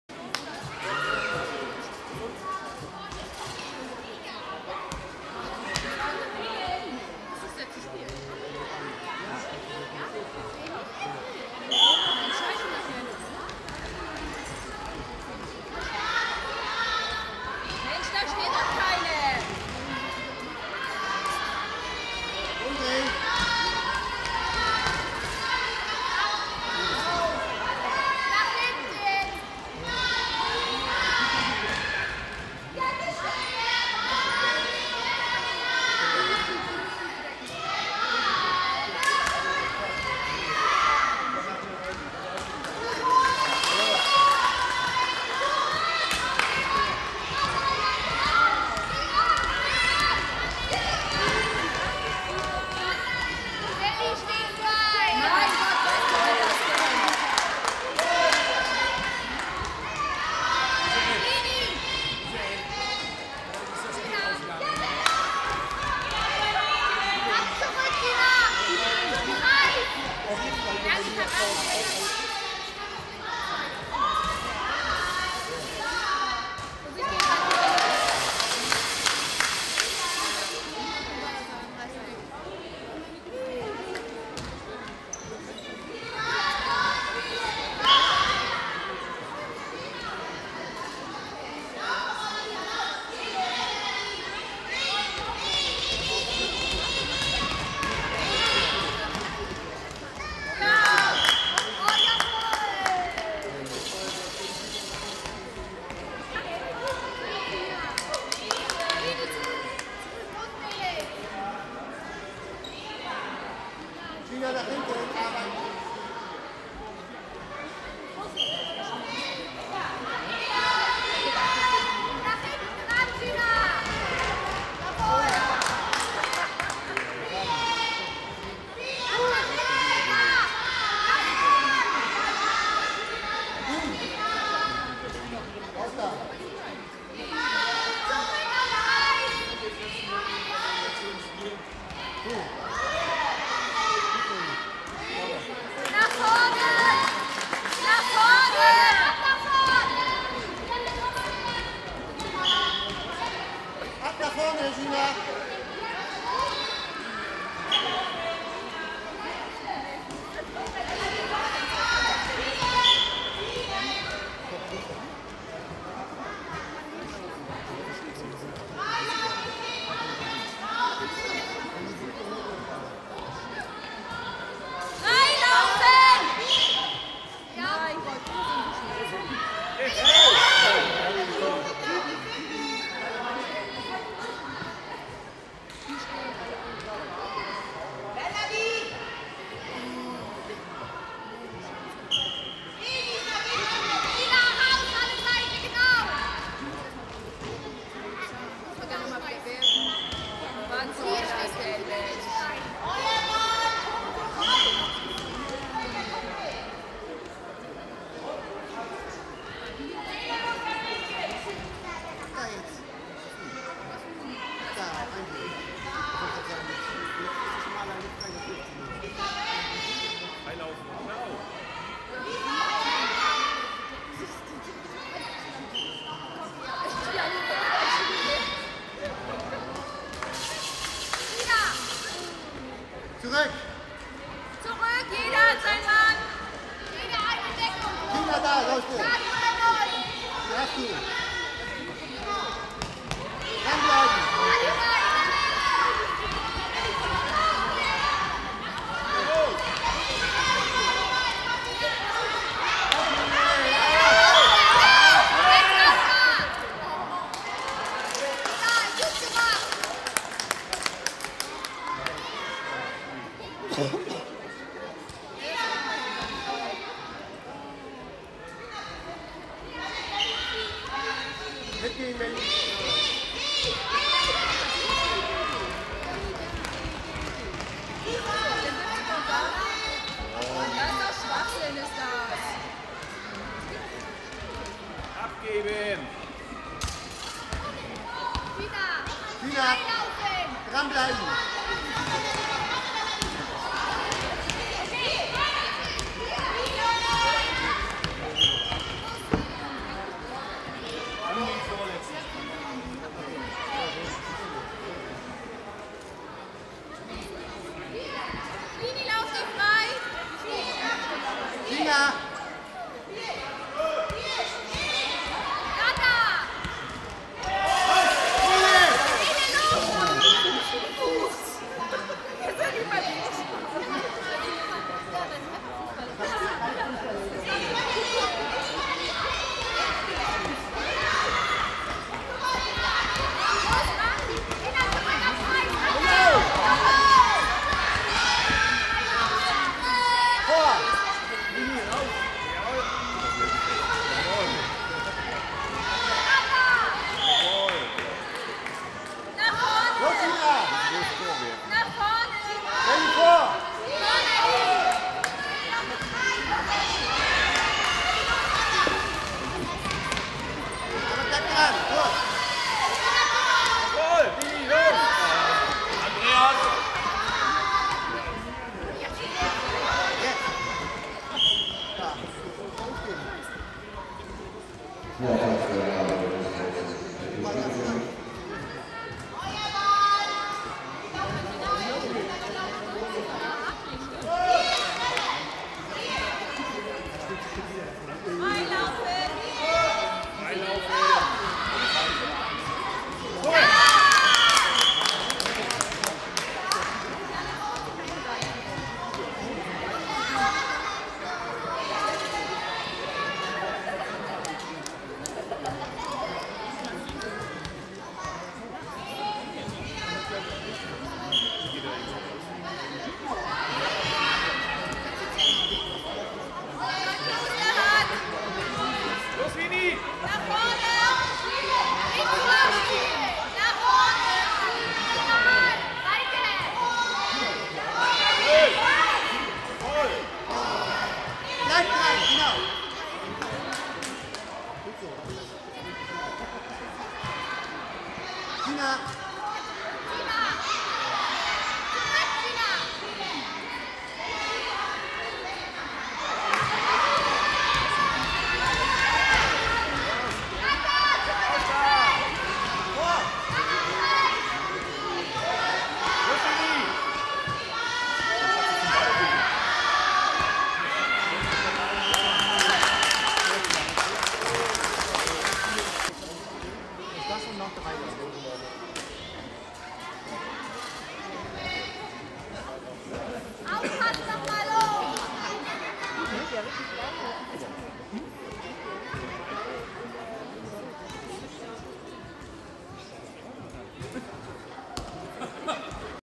15. Petermännchenturnier der F- Jugend 13. März 2010 - Vfl BW Neukloster vs. TSV Bützow
15. Petermännchenturnier der F- Jugend 13. März 2010 - Vfl BW Neukloster vs. TSV Bützow